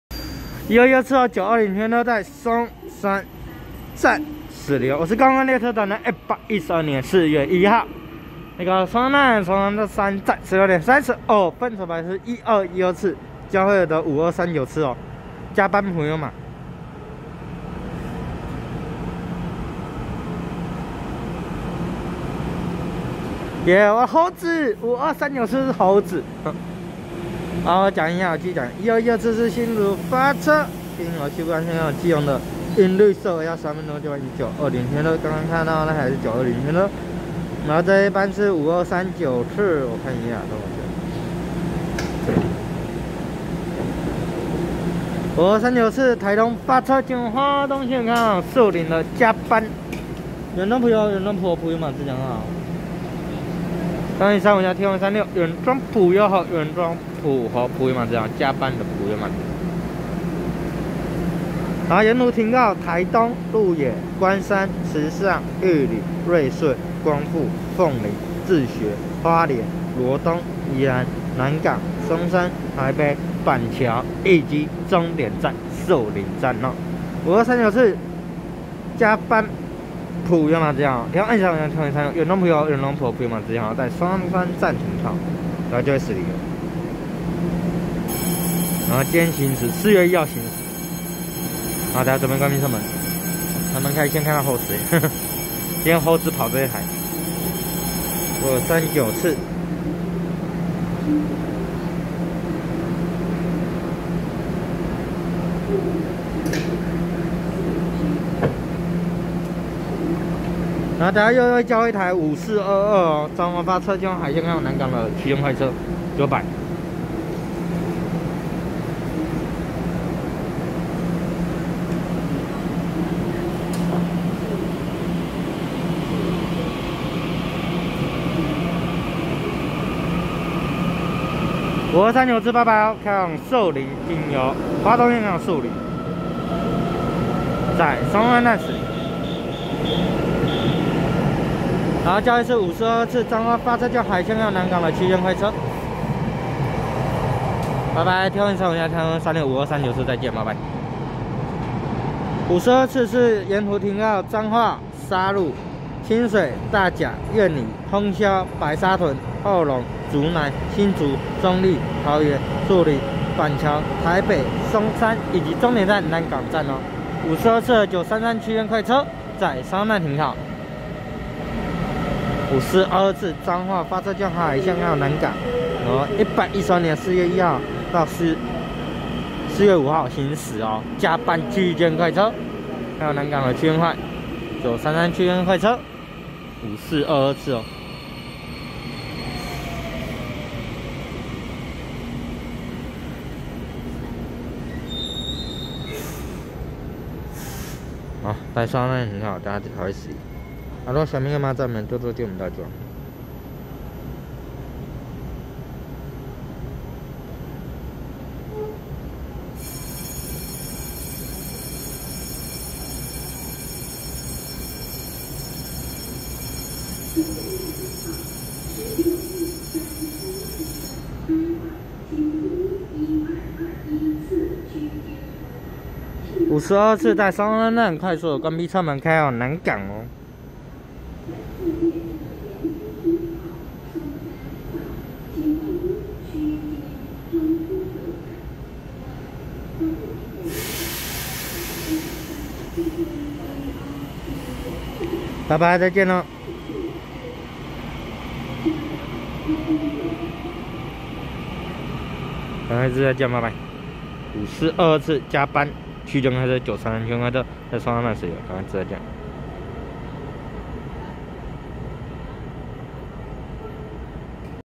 5239次台東發車經由花東線開往樹林的連假加班TEMU2035+TEMU2036原裝普悠猴原裝普猴普悠瑪自強號，5422次彰化發車經由海線開往南港的連假加班EMU933區間快車
112/4/1松山站16:33分拍攝1212次新竹發車經由西部幹線開往基隆的銀綠色微笑號EMU900型十輛一編組EMU920區間車，5239次台東發車經由花東線開往樹林的連假加班TEMU2035+TEMU2036原裝普悠猴原裝普猴普悠瑪自強號，5422次彰化發車經由海線開往南港的連假加班EMU933區間快車